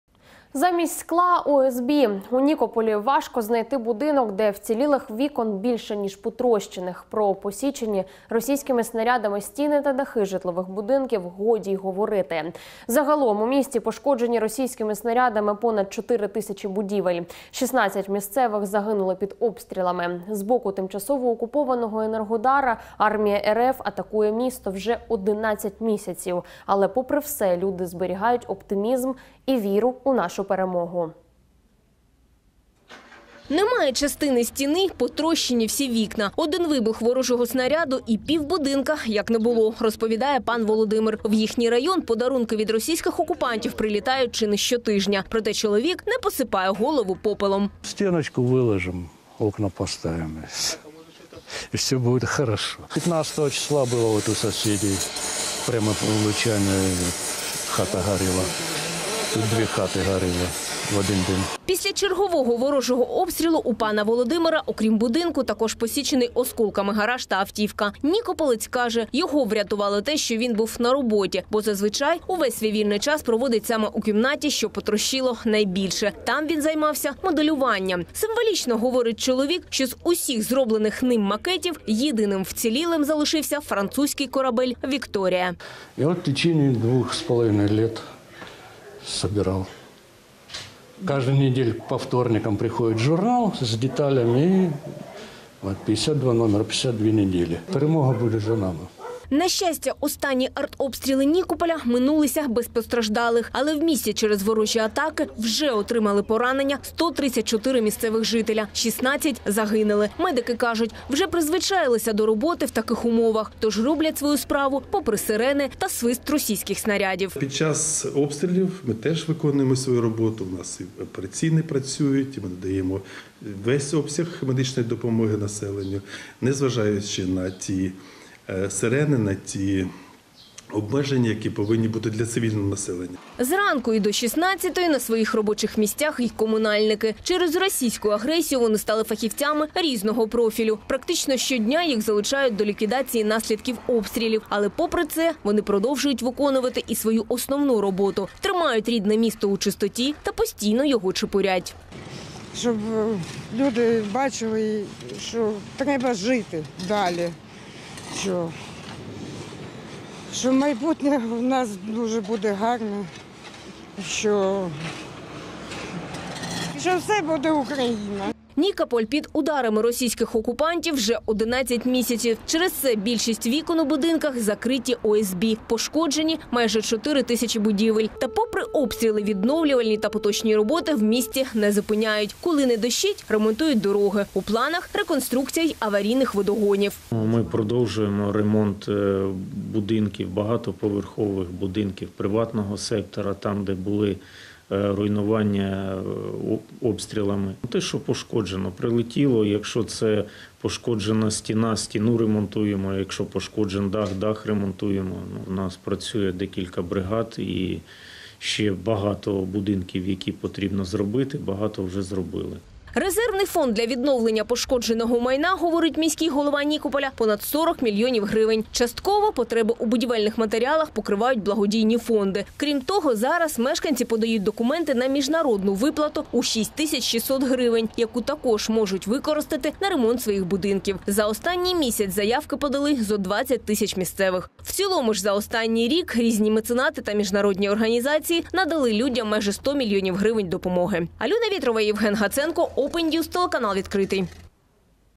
16 мешканців Нікополя загинули через обстріли російських окупантів
Замість скла - ОСБ. У Нікополі важко знайти будинок, де вцілілих вікон більше ніж потрощених. Про посічені російськими снарядами стіни та дахи житлових будинків, годі й говорити. Загалом у місті пошкоджені російськими снарядами понад 4 тисячі будівель. 16 місцевих загинули під обстрілами. З боку тимчасового окупованого Енергодара, армія рф атакує місто вже 11 місяців, але попри все люди зберігають оптимізм і віру у нашу Перемогу